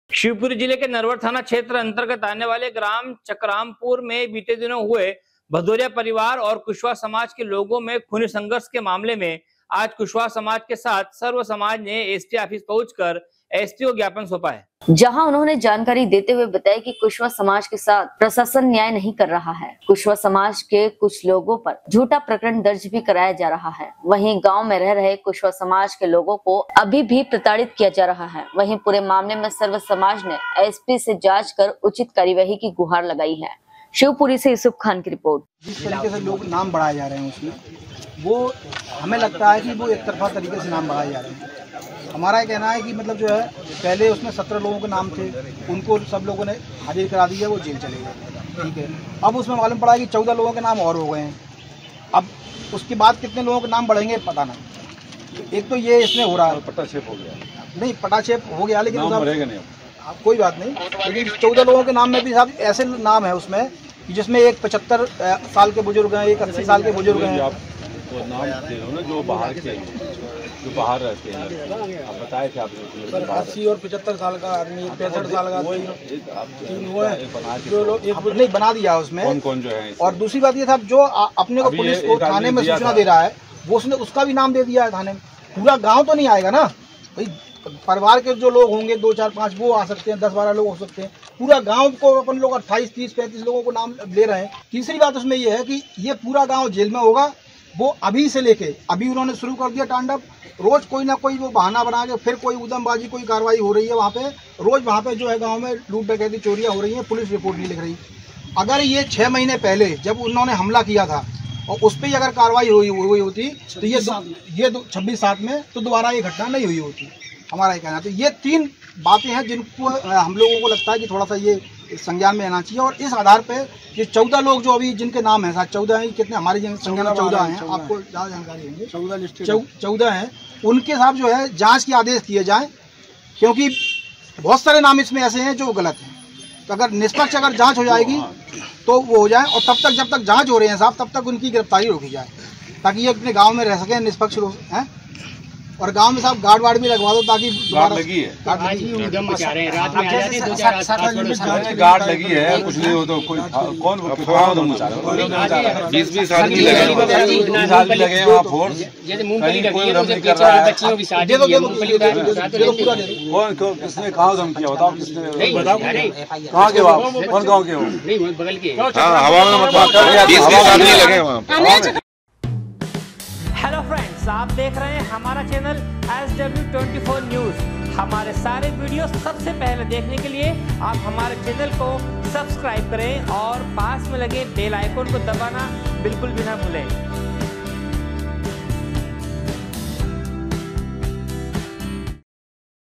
खूनी संघर्ष के मामले में आज कुशवाह समाज के साथ सर्व समाज ने एसपी ऑफिस पहुंचकर एसपी को ज्ञापन दिया है
शिवपुरी जिले के नरवर थाना क्षेत्र अंतर्गत आने वाले ग्राम चकरामपुर में बीते दिनों पूर्व हुए भदौरिया परिवार और कुशवाह समाज के लोगो में खूनी संघर्ष के मामले में आज कुशवाह समाज के साथ सर्व समाज ने एसपी ऑफिस पहुंचकर एसपी को ज्ञापन दिया है।
शिवपुरी से युसूफ खान की रिपोर्ट।